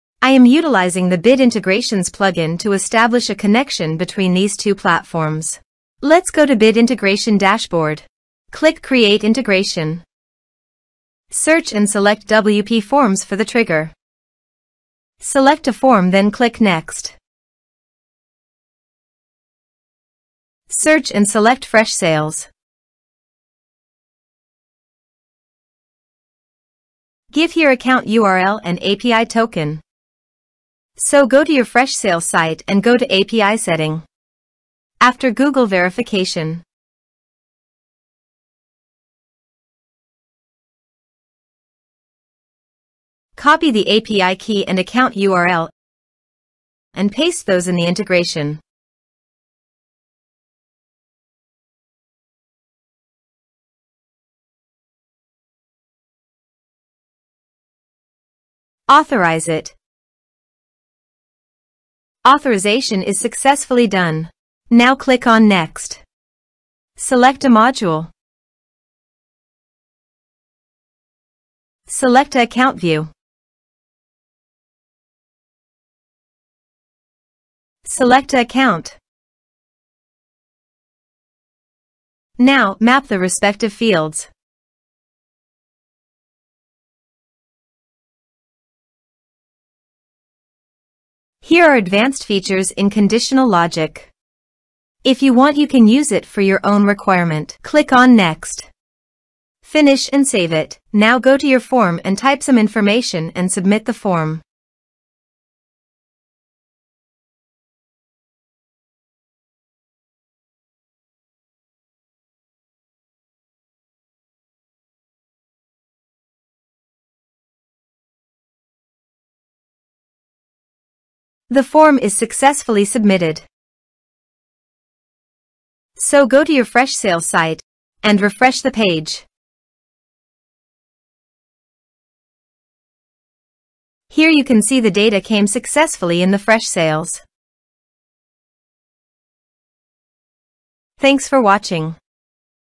(Free) Integrating WPForms with Freshsales | Step-by-Step Tutorial |Bit Integrations
🔗 Seamless Integration of WPForms with Freshsales CRM | Bit Integrations Tutorial

Welcome to our latest video tutorial! In this video, we'll guide you through the process of integrating WPForms with Freshsales CRM, all thanks to the powerful Bit Integrations WordPress plugin.

📌 *Freshsales CRM* is your go-to solution for customer relationship management. It's designed to supercharge your sales and marketing efforts by simplifying lead management, contact tracking, and more. Our tutorial will show you how to seamlessly connect WPForms with Freshsales CRM, unlocking a world of possibilities for your CRM experience.

**Key Highlights:**
✅ A step-by-step tutorial on integrating WPForms with Freshsales CRM using Bit Integrations.
✅ Elevate your sales and marketing by streamlining lead management and contact tracking.
✅ Experience the power of seamless data integration to optimize your business processes.

Elevate your customer engagement with Fluent Forms and Benchmark Email integration! 👍